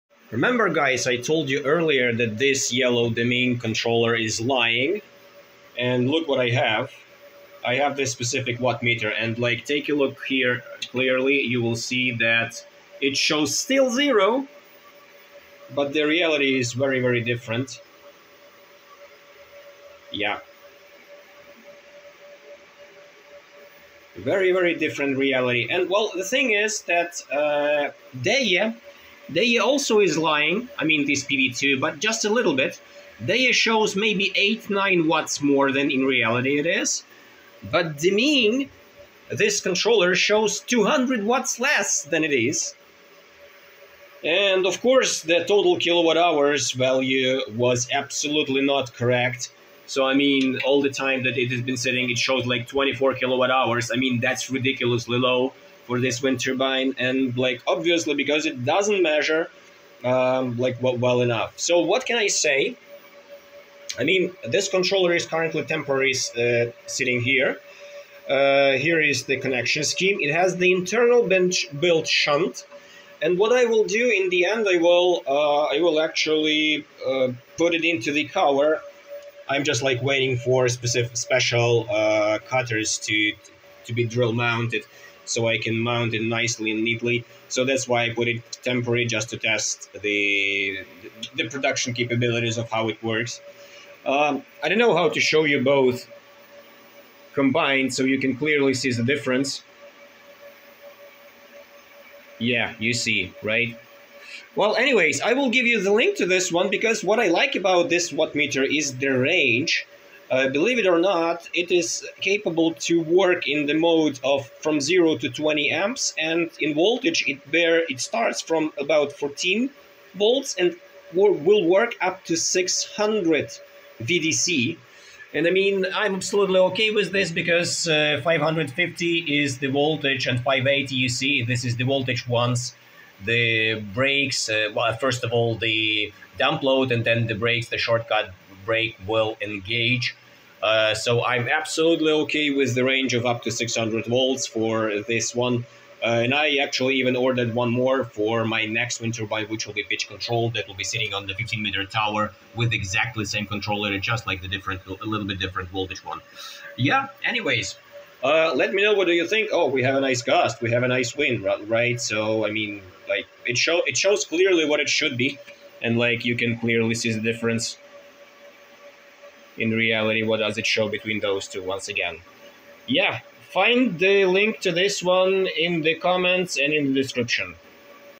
Found a great wattmeter, sharing some experience
Link to this wattmeter
Select a version: D69-3062 shunt.